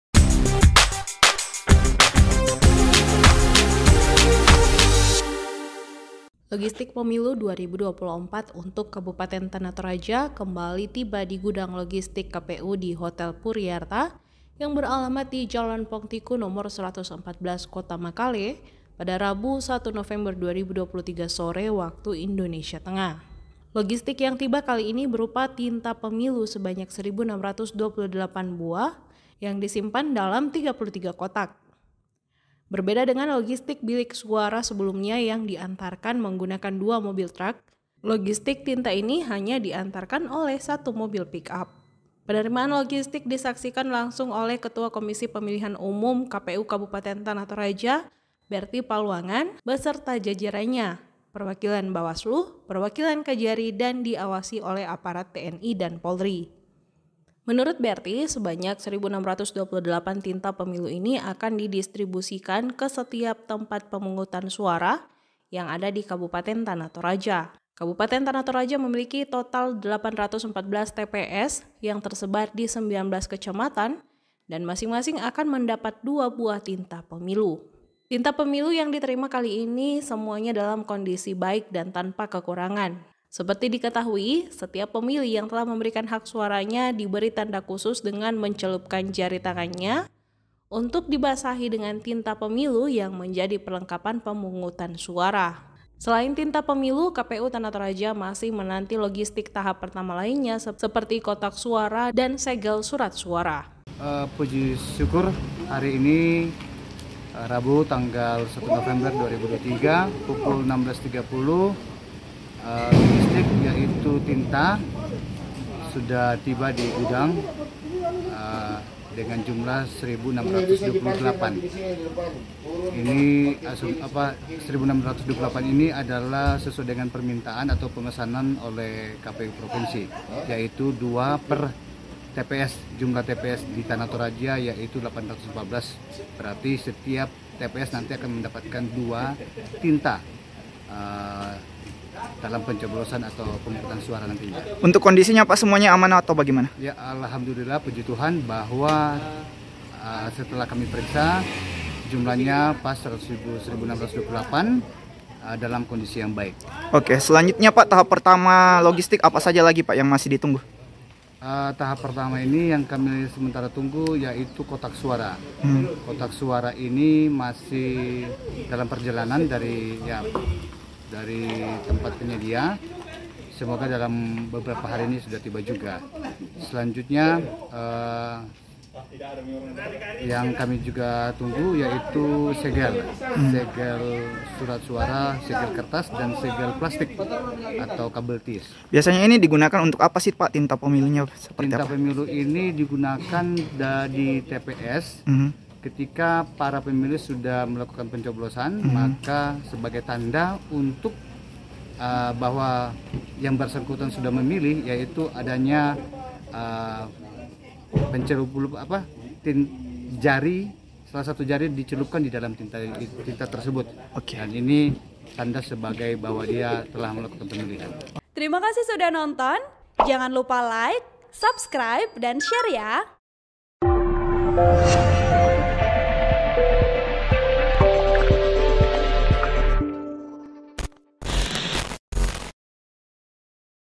Tinta Pemilu 2024 Tiba di Gudang Logistik, KPU Tana Toraja : Akan Dibagikan Dua Untuk Tiap TPS
TRIBUNTORAJA.COM, MAKALE - Logistik Pemilu 2024 untuk Kabupaten Tana Toraja kembali tiba di gudang logistik KPU di Hotel Puri Artha, Jalan Pongtiku Nomor 114, Kota Makale, Rabu (1/11/2023) sore Wita.

Logistik yang tiba kali ini berupa tinta Pemilu sebanyak 1.628 buah yang disimpan dalam 33 kotak.

Berbeda dengan logistik bilik suara sebelumnya yang diantarkan menggunakan dua mobil truck, logistik tinta ini hanya diantarkan oleh satu mobil pik-up dengan nomor polisi B 9491 PAJ.

Penerimaan logistik disaksikan langsung oleh Ketua Komisi Pemilihan Umum (KPU) Kabupaten Tana Toraja, Berthy Paluangan, beserta jajarannya, perwakilan Bawaslu, perwakilan Kajari, dan diawasi oleh aparat TNI dan Polri.

“Puji syukur hari ini Rabu tanggal 1 November 2023 pukul 16.30 Wita, logistik yaitu tinta sudah tiba di gudang dengan jumlah 1.628 buah,” kata Berthy kepada Tribun Toraja di lokasi.

Menurut Berthy, sebanyak 1.628 tinta Pemilu ini akan didistribusikan ke setiap tempat pemungutan suara (TPS) yang ada di Kabupaten Tana Toraja.

Kabupaten Tana Toraja memiliki total 814 TPS yang tersebar di 19 kecamatan dan masing-masing akan mendapat dua buah tinta Pemilu.

“Jadi, 1.628 ini sesuai dengan permintaan ataupun pemesanan oleh KPU Provinsi Sulsel. Jumlah TPS di Tana Toraja yaitu 814 TPS, berarti setiap TPS nanti akan mendapatkan dua tinta dalam pencoblosan atau pemungutan suara nantinya,” papar Berthy.

Tinta Pemilu yang diterima kali ini semuanya dalam kondisi baik dan tanpa kekurangan kata Berthy.

“Alhamdulillah, Puji Tuhan, bahwa setelah kami periksa jumlahnya pas 1.628 dan dalam kondisi yang baik.”

Seperti diketahui, setiap pemilih yang telah memberikan hak suaranya diberi tanda khusus dengan mencelupkan jari tangannya untuk dibasahi dengan tinta Pemilu yang menjadi perlengkapan pemungutan suara.

Selain tinta Pemilu, KPU Tana Toraja masih menanti logistik tahap pertama lainnya berupa kotak suara dan segel surat suara.

“Tahap pertama ini yang sementara kami tunggu yaitu kotak suara. Kotak suara ini masih dalam perjalanan dari tempat penyedia, semoga dalam beberapa hari ini sudah tiba juga,” lanjut Berthy.

“Selanjutnya yang kami juga tunggu yaitu segel surat suara, segel kertas, dan segel plastik atau kabel tis,” tandasnya. (*)

Artikel ini telah tayang di Tribuntoraja.com dengan judul Tinta Pemilu 2024 Tiba di Kabupaten Tana Toraja, Ketua KPU: Satu TPS Dapat Dua Buah,

Penulis: Muhammad Rifki | Editor: Apriani Landa
VP : Febe M. Tolan